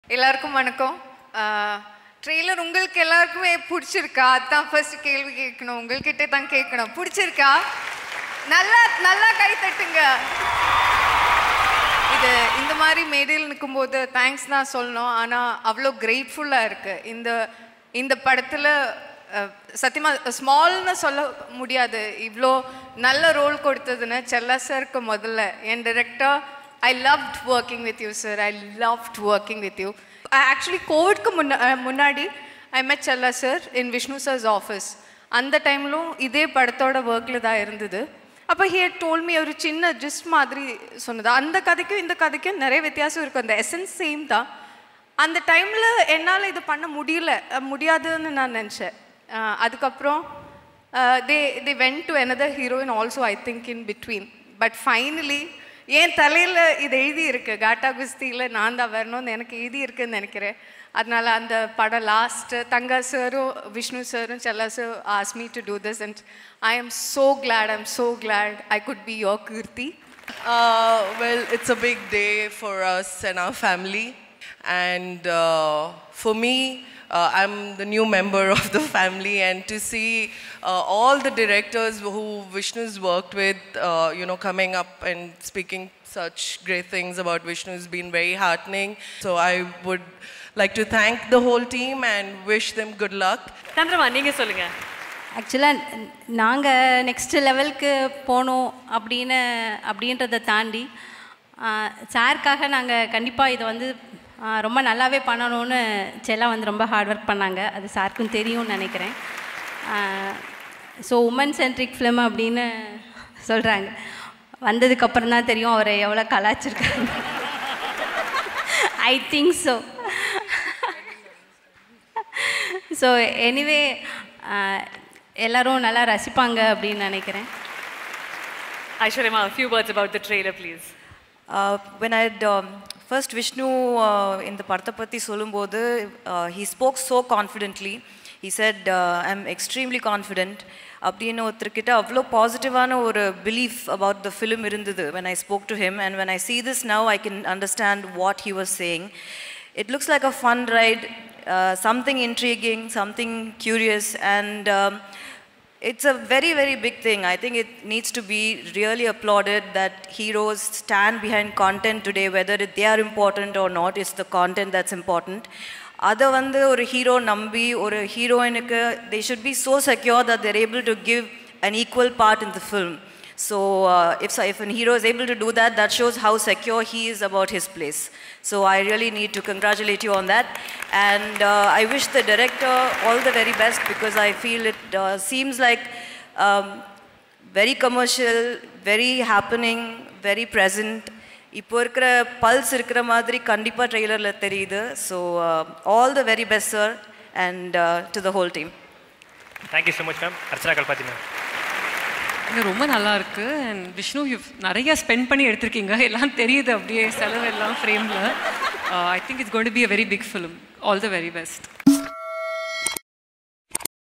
Women Centric Movie-னு சொல்றாங்க - Aishwarya Lekshmi | Gatta Kusthi Audio Launch | Vasanth TV
Women Centric Movie-னு சொல்றாங்க - Aishwarya Lekshmi Speech | Gatta Kusthi Audio & Trailer Launch | Vishnu Vishal | Vasanth TV

கட்டா குஸ்தி திரைப்படத்தின் ஆடியோ மற்றும் டிரெய்லர் வெளியீட்டு விழா | Audio & Trailer Launch Of Gatta Kusthi |